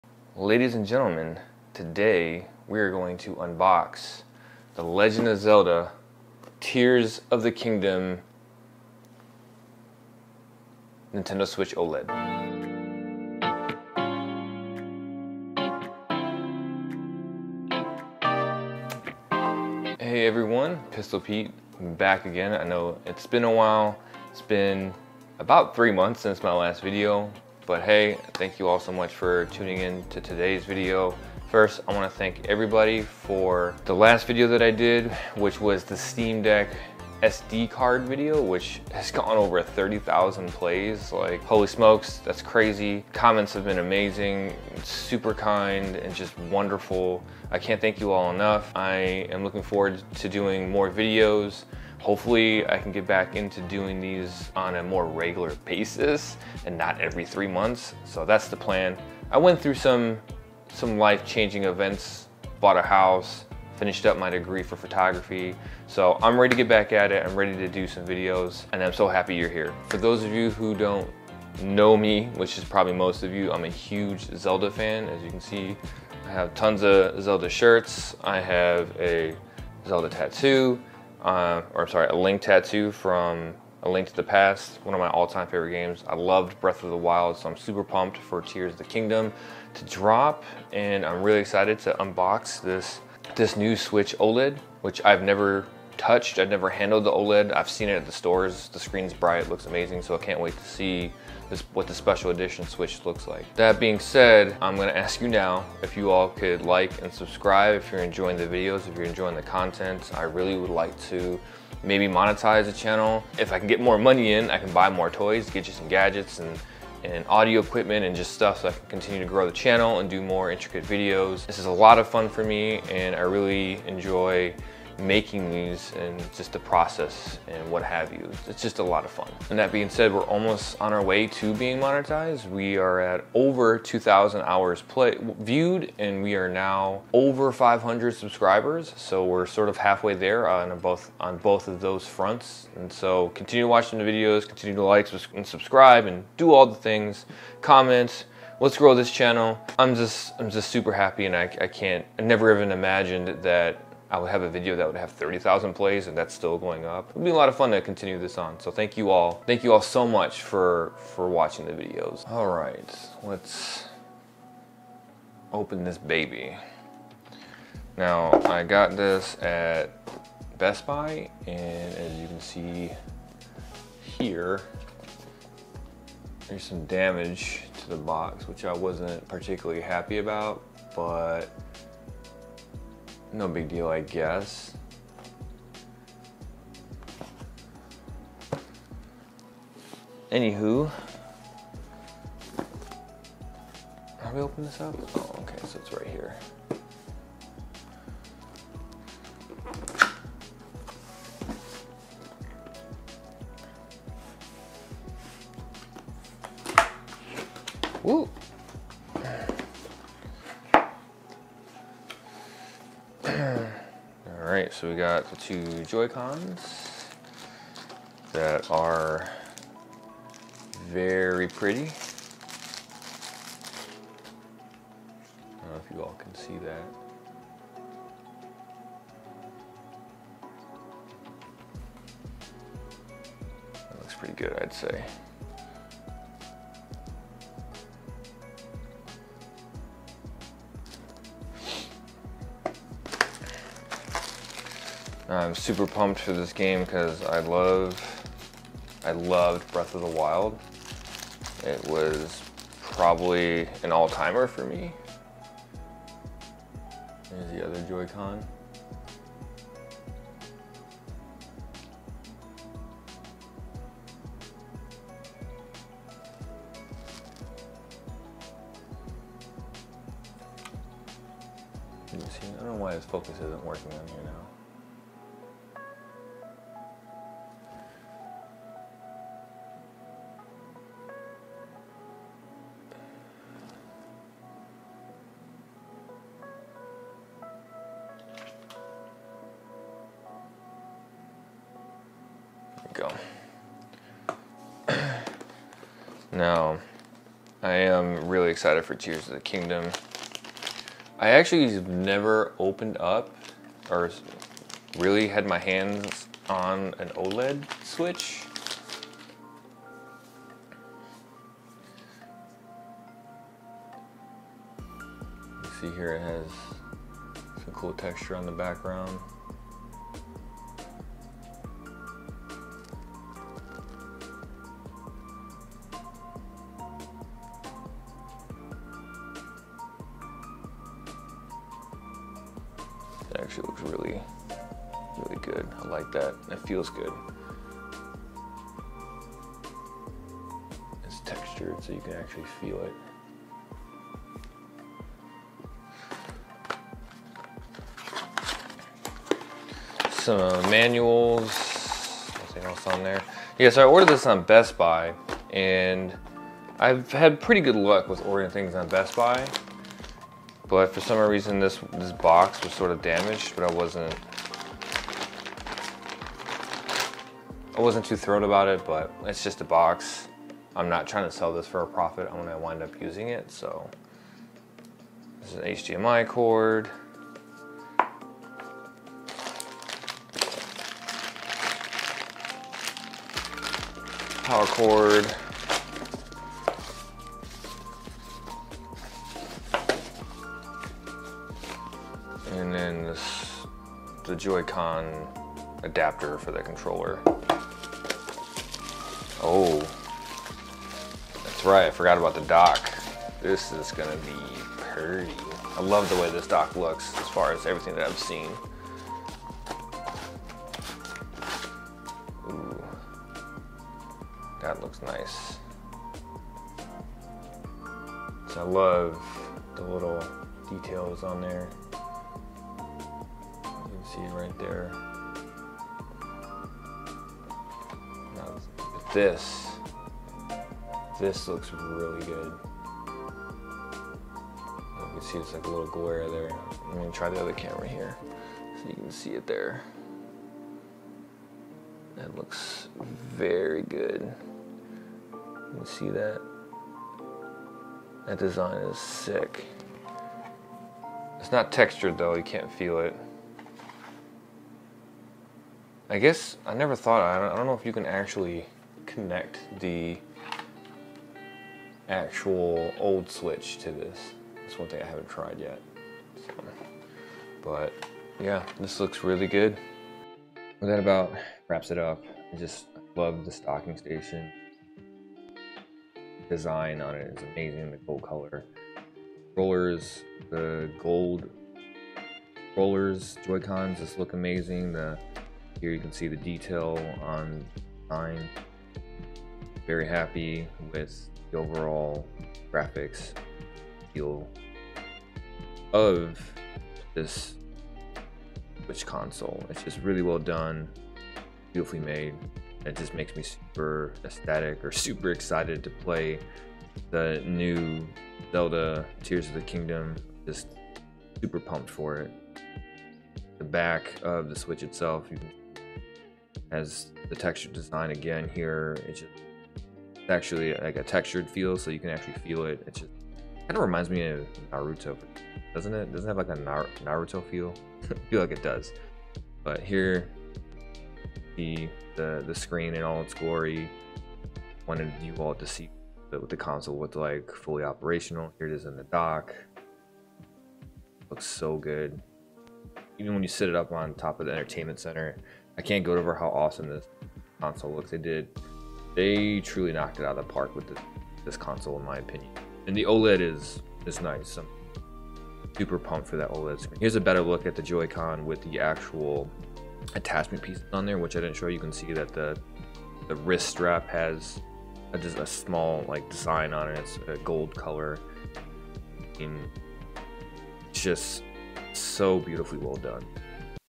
UNBOXING Legend of Zelda: Tears of the Kingdom Special Edition OLED Switch
New Twitch schedule starting this Sunday 5/7 - Sunday, Monday, Wednesday 7pm EST - 12pm EST

I'm back! Quick little unboxing video of the Nintendo Switch OLED Special Edition Legend of Zelda: Tears of the Kingdom

0:00 Opening
00:18 Intro and channel update
2:43 Unboxing
7:20 Close up shots and closing